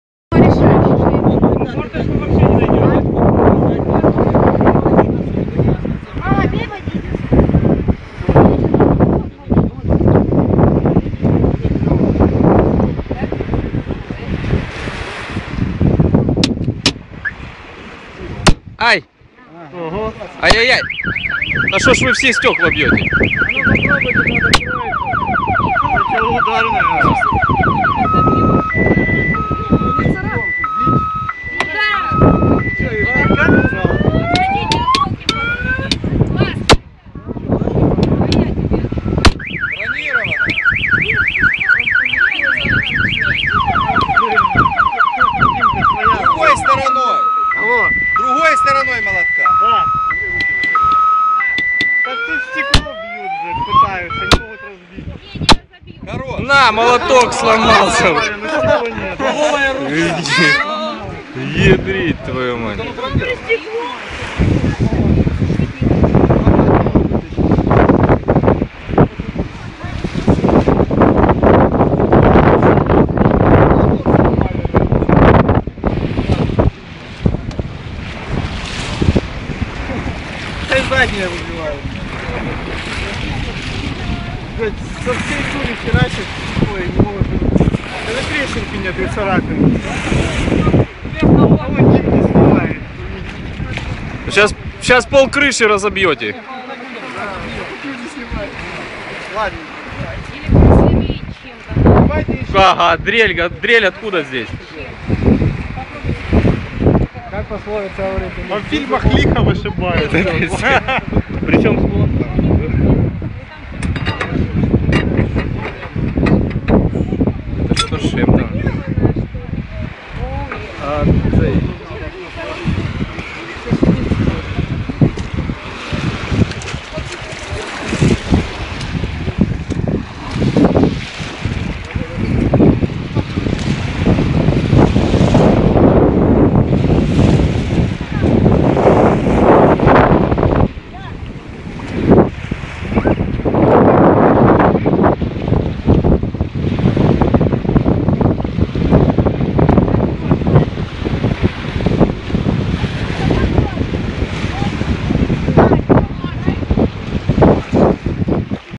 Таурег против молотка !. Забавный случай в Украине
Украинцы не смогли открыть таурег и им на помощь пришел молоток! Но и он им не помог, и они не смогли разбить стекло!:)

Ураина, крым, таурег vs молоток, прикол 2014,2015, смешно!